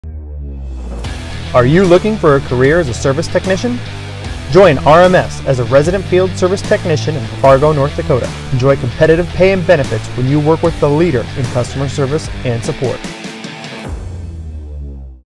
Join TeamRMS as a Resident Field Service Technician in Fargo, ND
Are you looking for a career as a service technician. Join RMS as a Resident Field Service Technician in Fargo, ND. Enjoy competitive pay and benefits when you work with the leader in customer service and support. today!